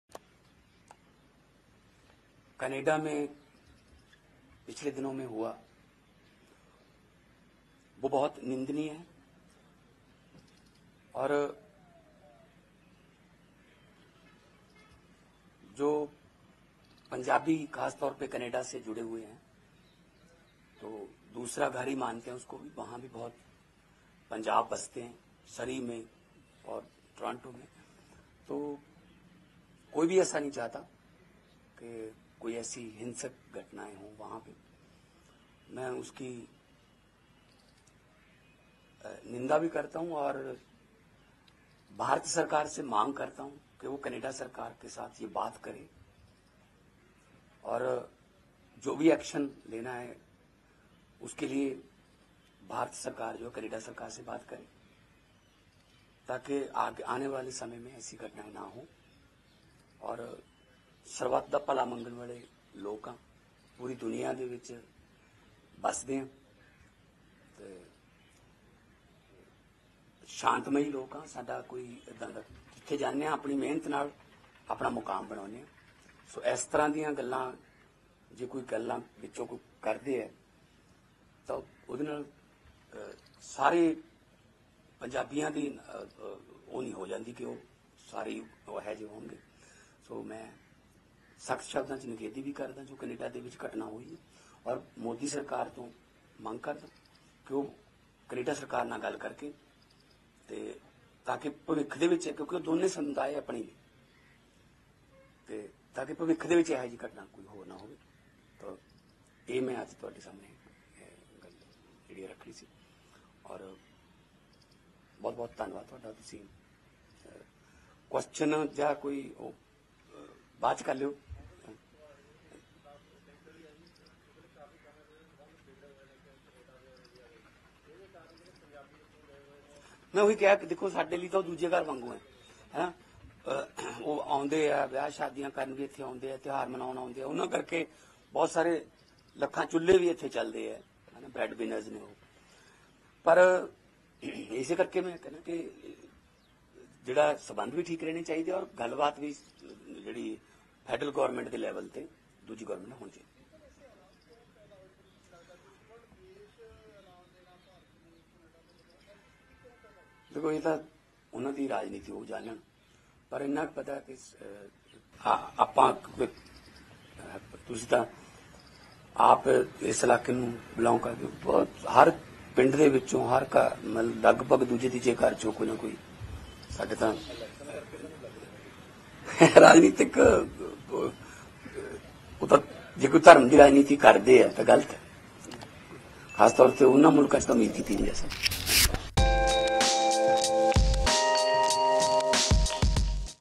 'What happened in Canada in the past few days is highly condemnable,' Punjab CM Bhagwant Mann
'What happened in Canada in the past few days is highly condemnable. Punjabis, especially those related to Canada, consider it their second home. Punjabis reside there in Surrey and Toronto. So, nobody wants such incidents of violence there. I condemn it.' said Punjab CM Bhagwant Mann